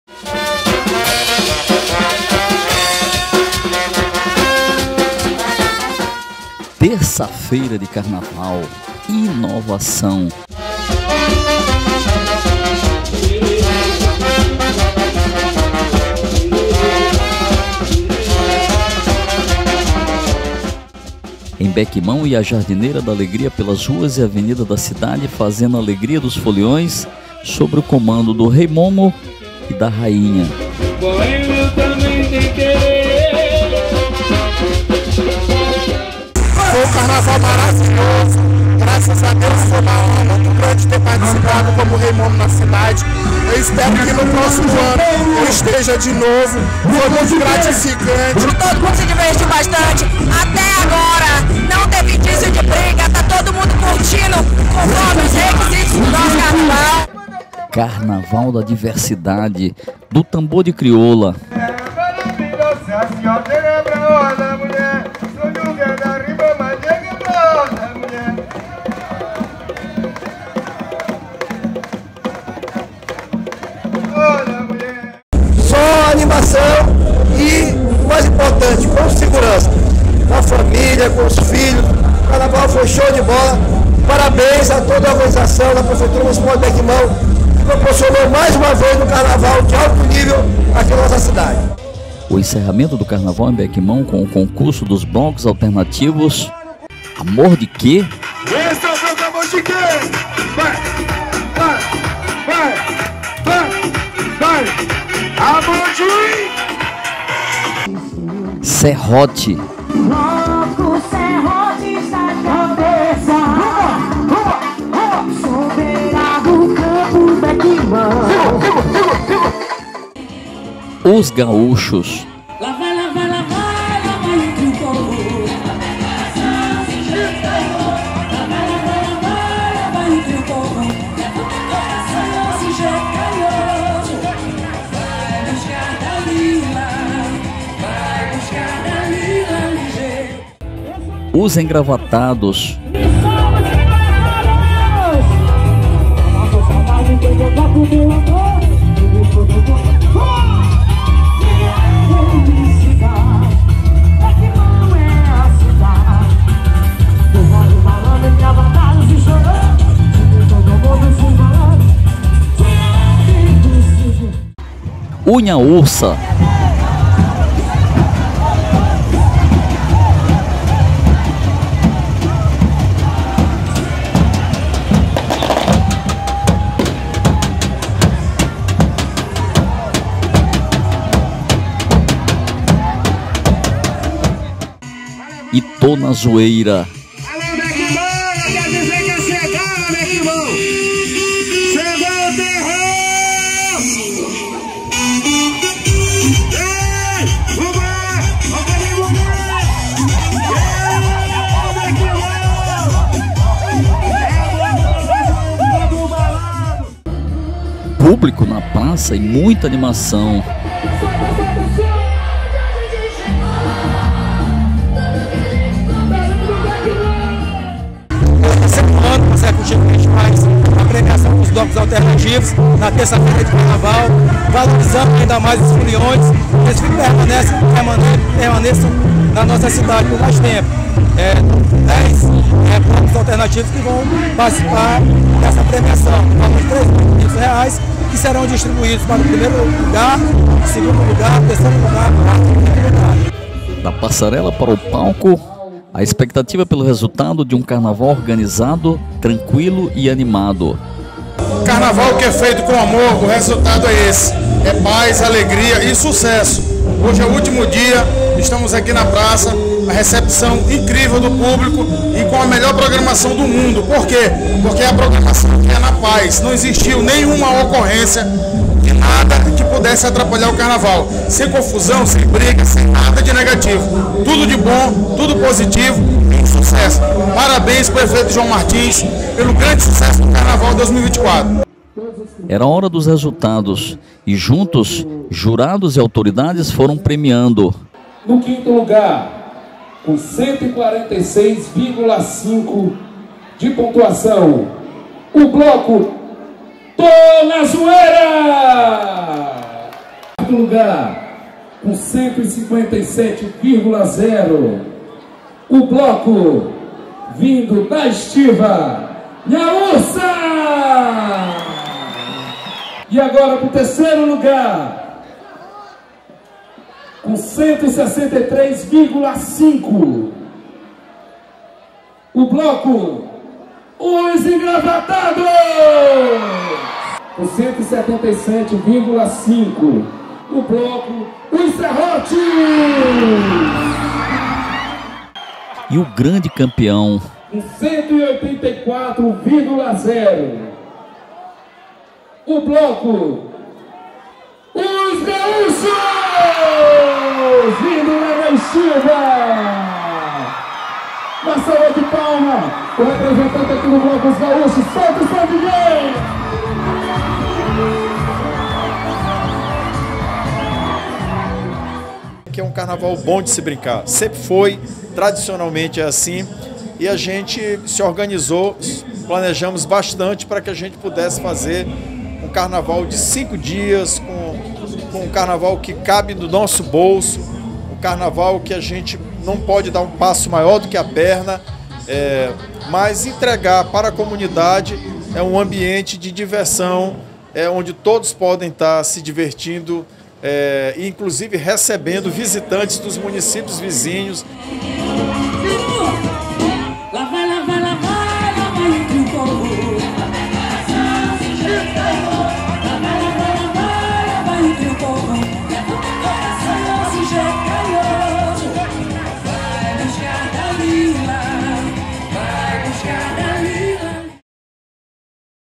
Carnaval da Nossa Gente de Bequimão do Prefeito João Martins, é sucesso com a terça da diversidade
Carnaval da Nossa Gente 2024 de Bequimão do Prefeito João Martins, é sucesso com a terça da diversidade